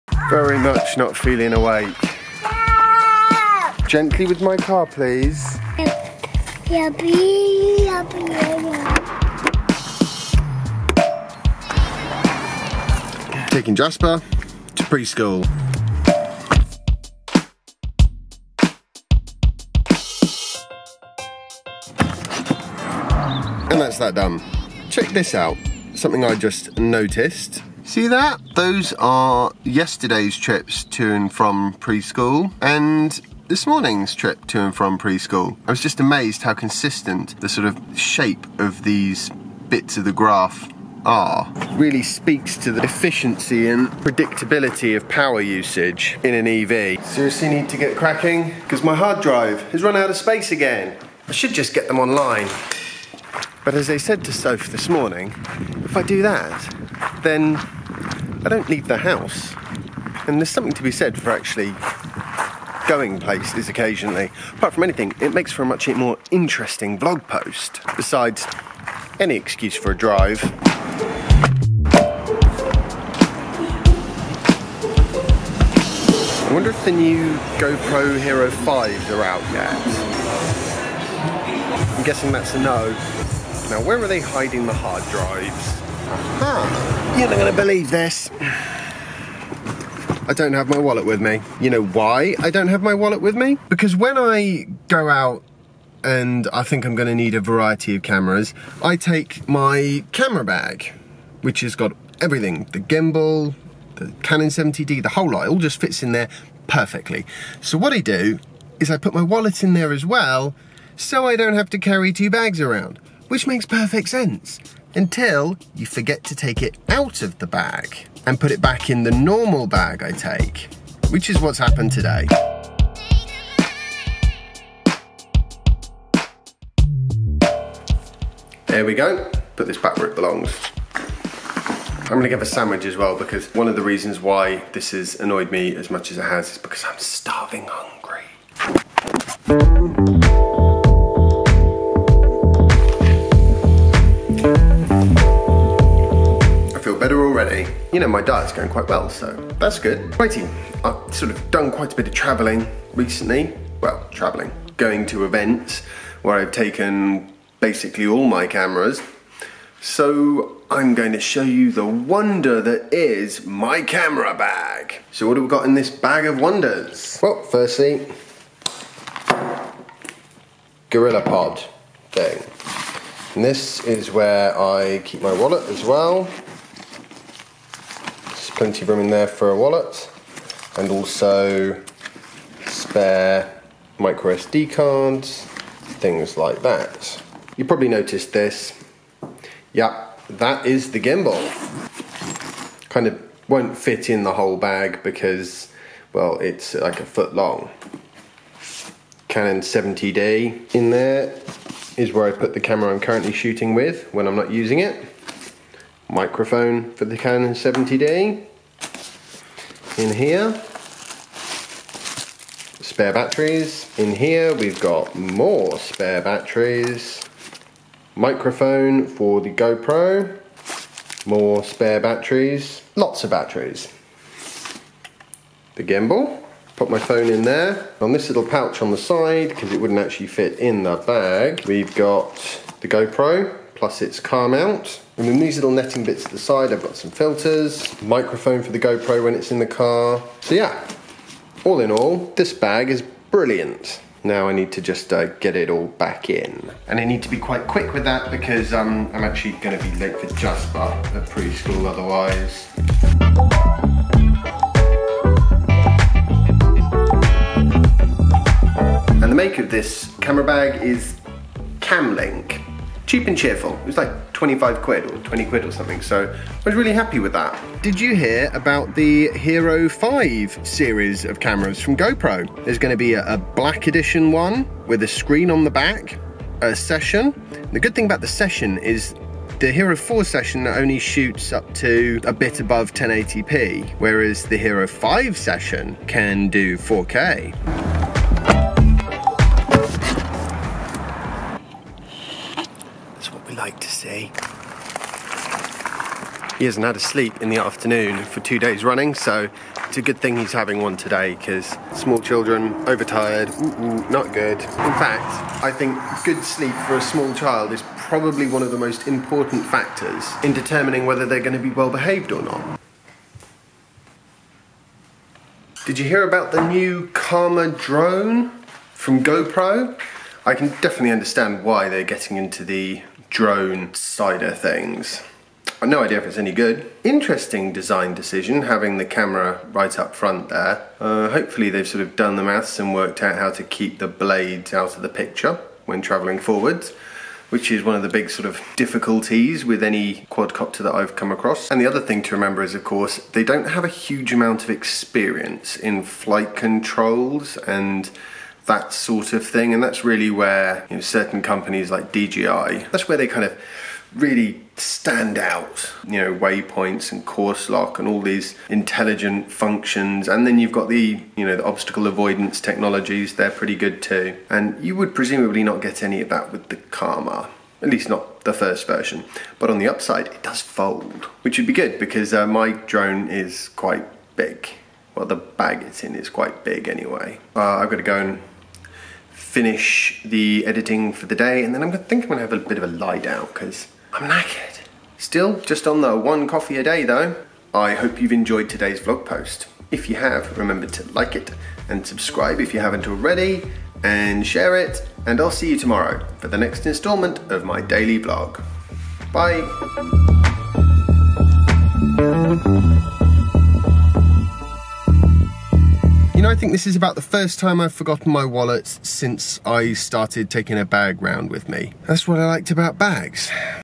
Bag of Wonders
When I'm out and about, what do I use to keep my cameras safe? Also GoPro have released their next generation of cameras, including one that flies!